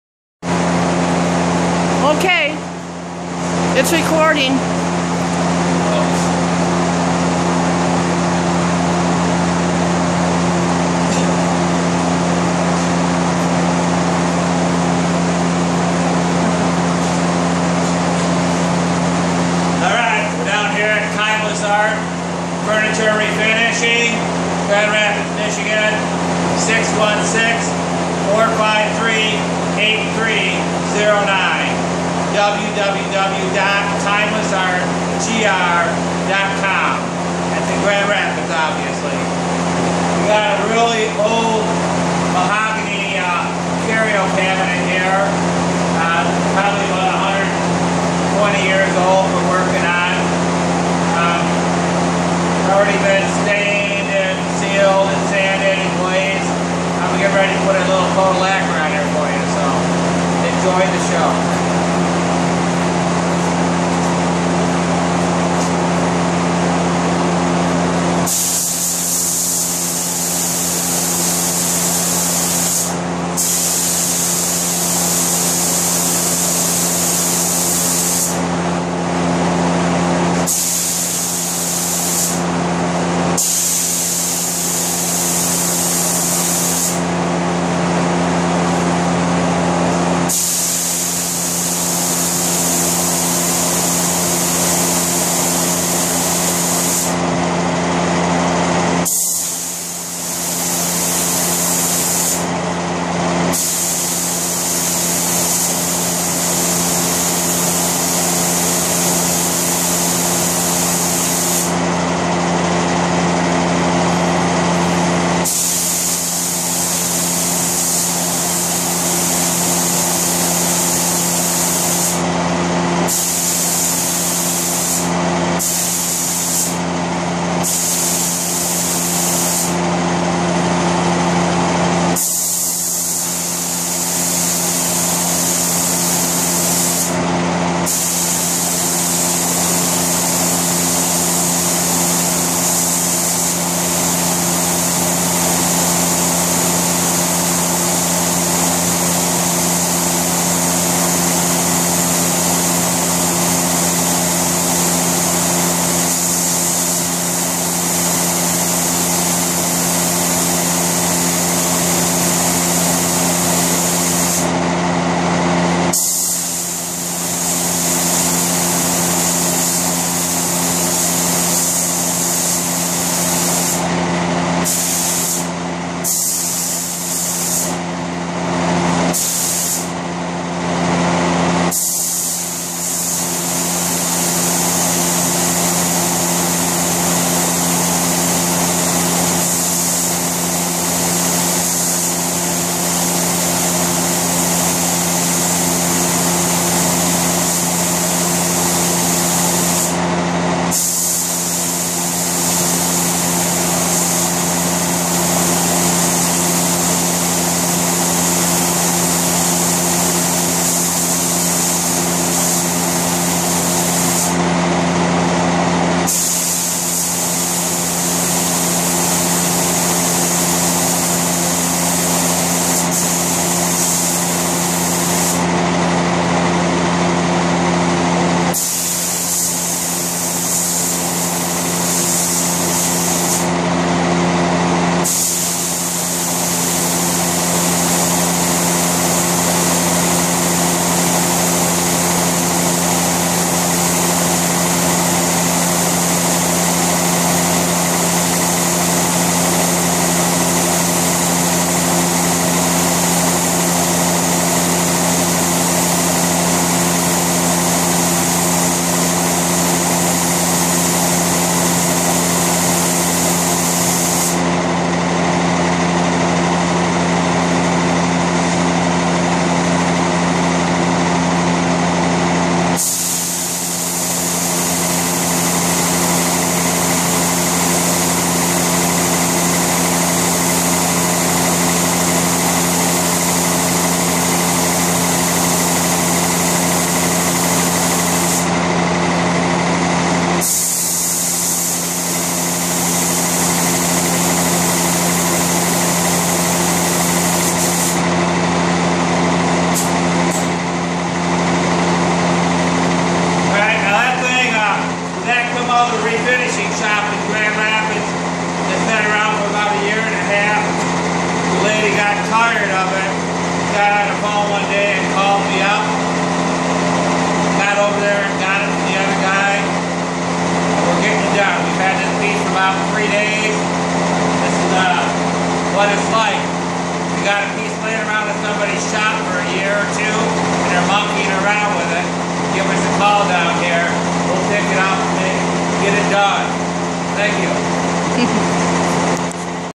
antique curio cabinet refinishing timeless arts refinishing
timeless arts refinishing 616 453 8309 grand rapids mi .shows a 100 year old mahogany curio cabinet that we had restored .the back legs were cut off ,,bruce had to replace the missing parts of the legs .timeless arts refinishing, can restore and refinish your fine furniture,antiques, and antique radios ,oriental furniture.